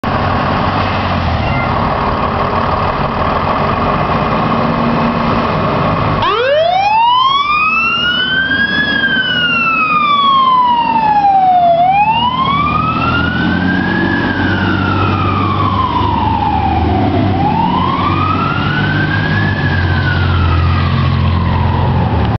F.E.F.D ER-271 Responding To A Reported Structure Fire!!!
We are AMA {automatic mutual aid} with hudson falls for any calls during the day 6 am - 6pm